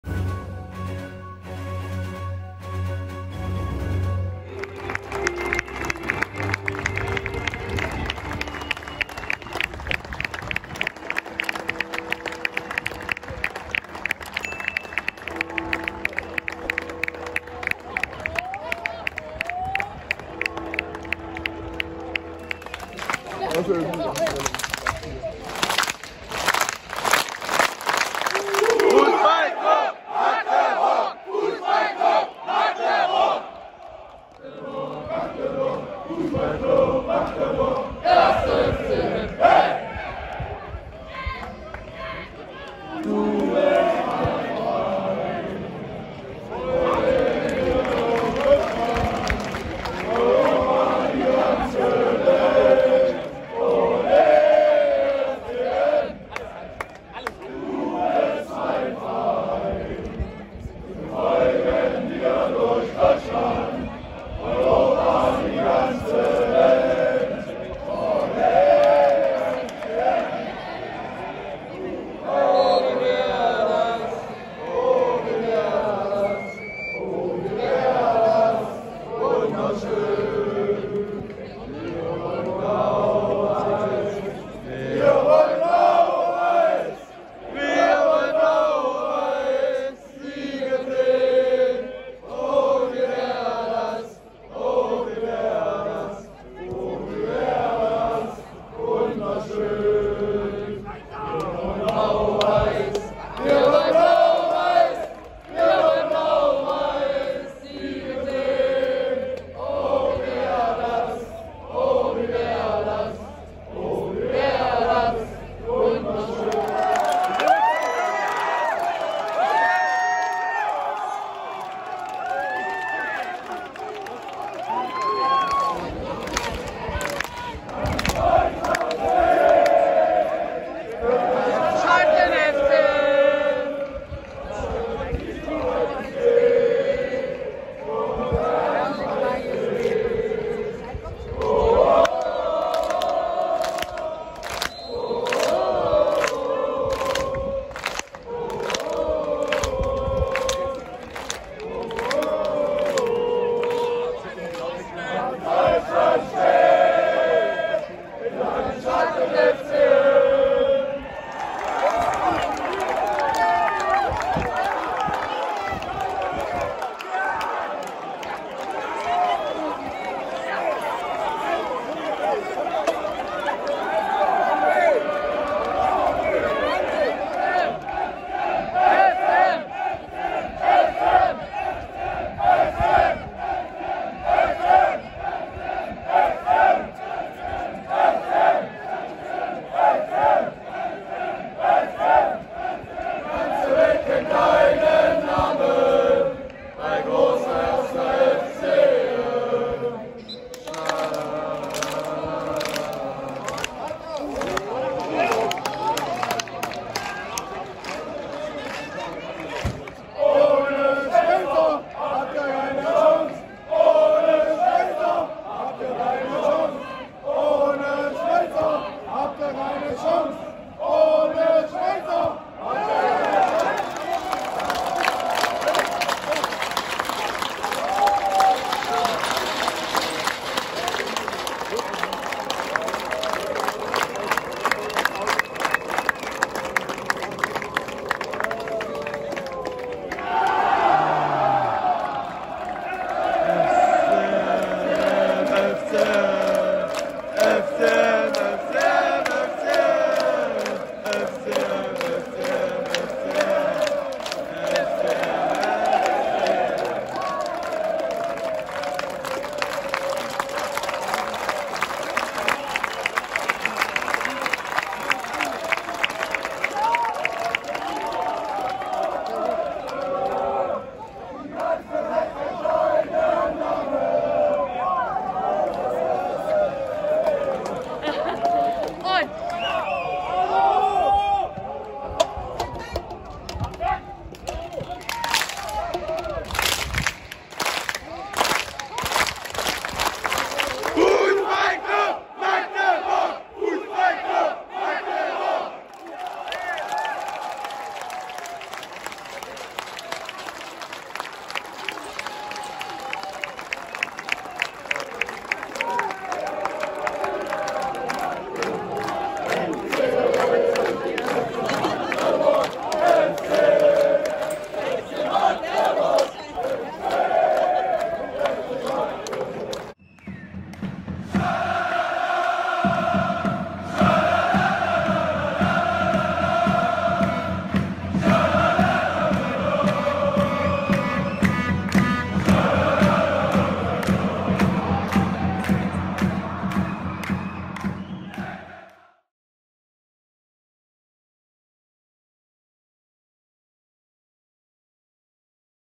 BORUSSIA DORTMUND II gegen 1.FC MAGDEBURG (0:2) Von Fans für Fans - Emotionen pur | 11.09.2021
BORUSSIA DORTMUND II 0:2 1.FC MAGDEBURG

🏆| 3.Liga Saison 21/22 - 8.Spieltag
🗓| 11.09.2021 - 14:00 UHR
🏟| Stadion Rote Erde (Dortmund)
🗣| 1.260 - ca. 500 Gäste